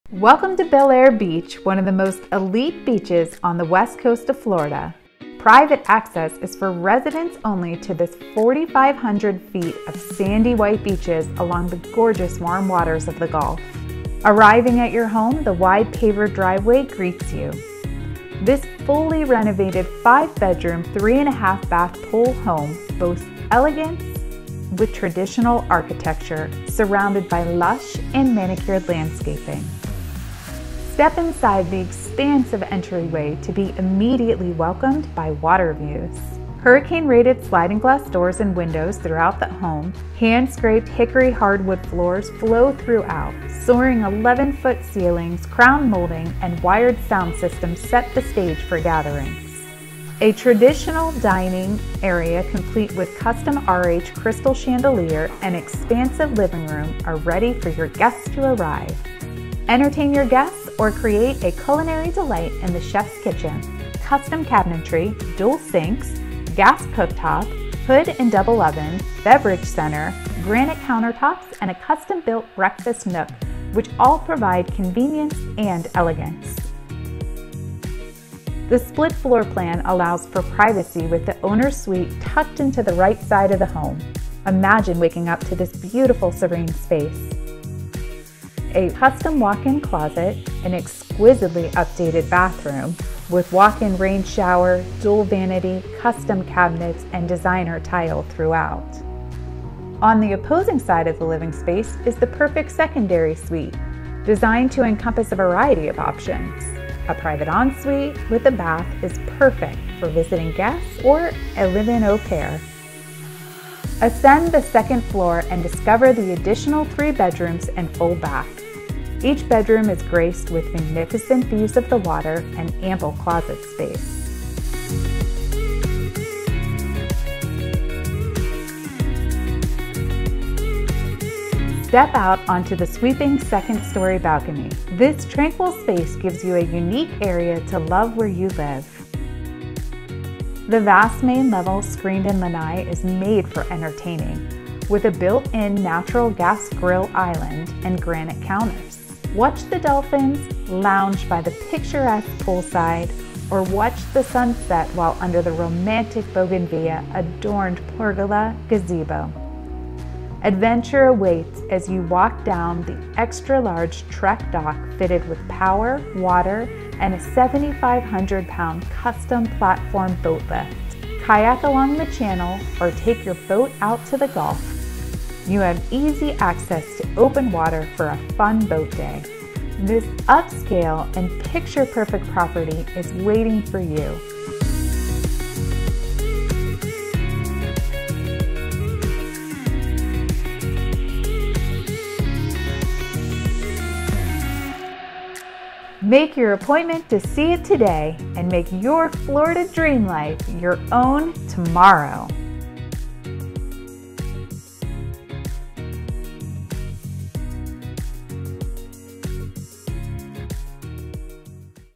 611 Belle Isle - Walkthrough
This Belleair Beach beauty gives you a tranquil space to relax and unique area to love where you live!